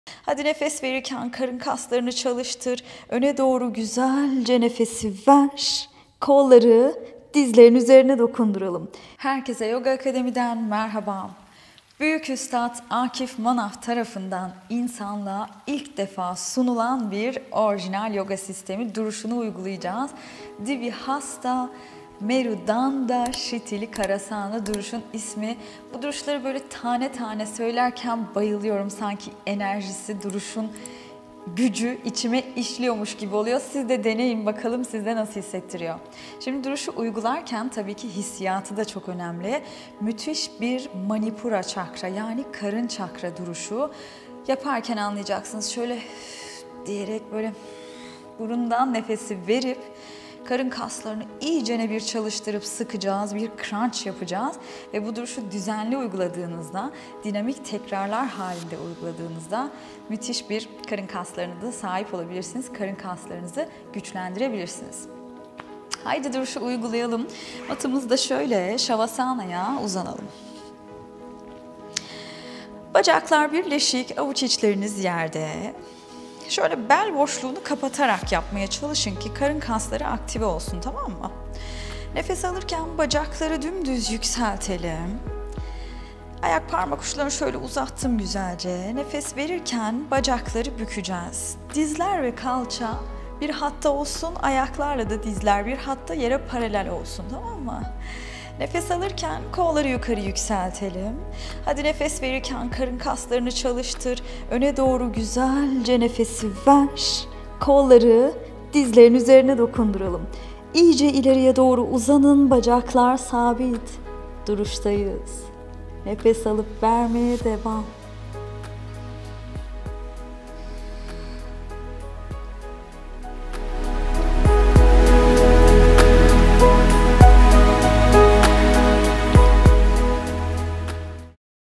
İnsanlığa İlk Defa Sunulan Teknik II Dvihasta Merudanda Şithilikaranasana II Nava Nur Tüzüner
Uluslararası Yoga Federasyonu Başkanı Akif Manaf tarafından insanlığa ilke defa sunulan Orijinal Yoga Sistemi tekniği, bu haftanın duruşu: Dvihasta Merudanda Şithilikaranasana. Tam bir crunch duruşu! :) Karın kasları ve bu bölgedeki kaslar çok etkili bir şekilde çalışıyor. Duruşu dinamik uygulaması ayrı, statik kalması ayrı güçlü bir etkiye sahip.  İyi güçlenmeler dileriz! :)